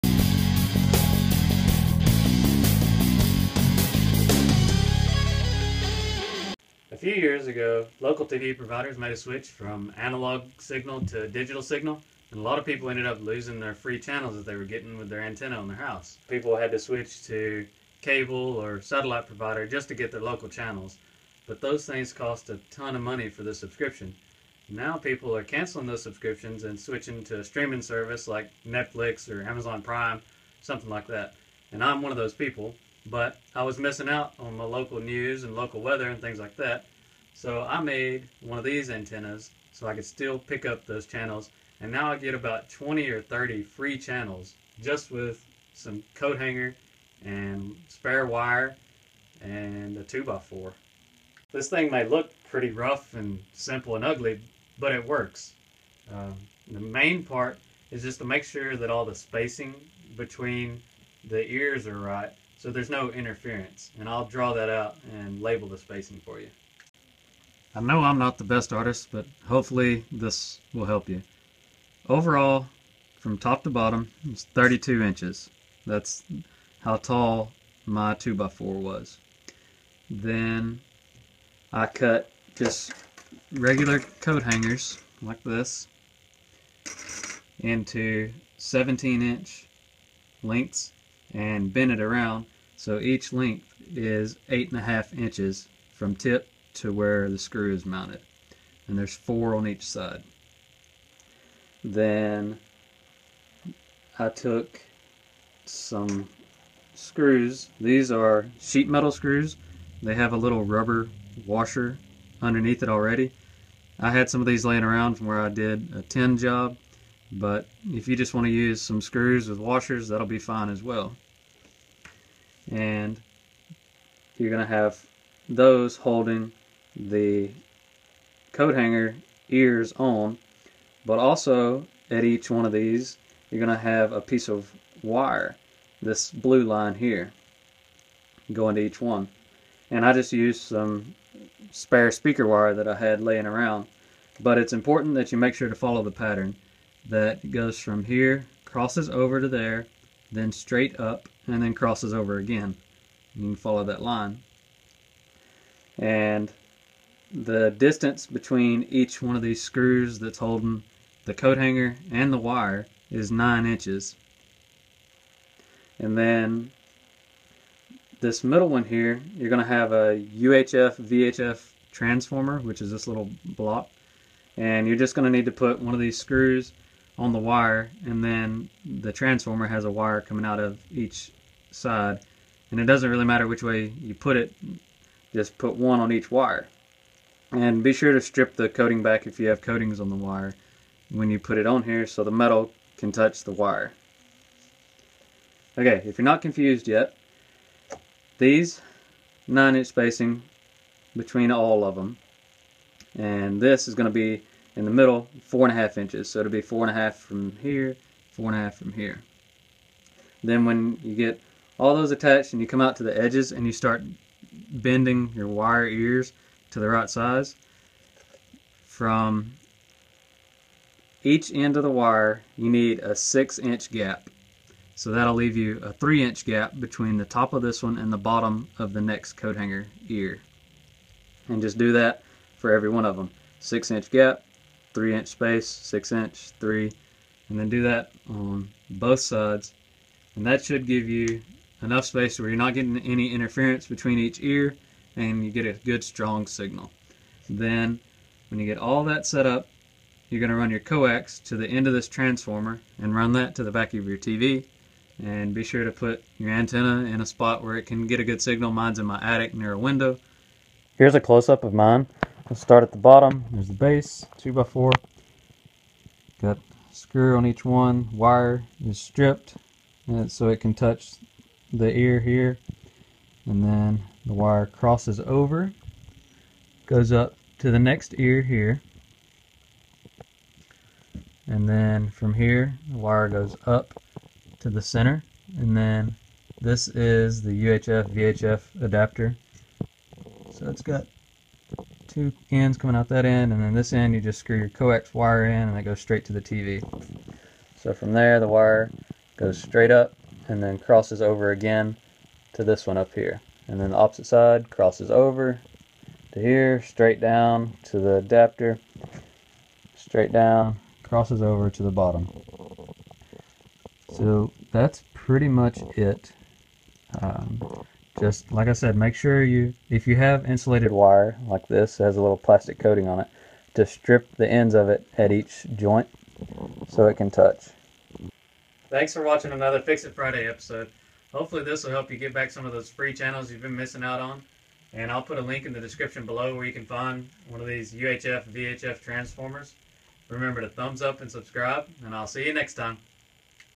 Fix-It Friday: Cheap Digital TV Antenna
Homemade TV antenna to pick up digital signal from local tv providers. Get your free channels back!

UHF/VHF Transformer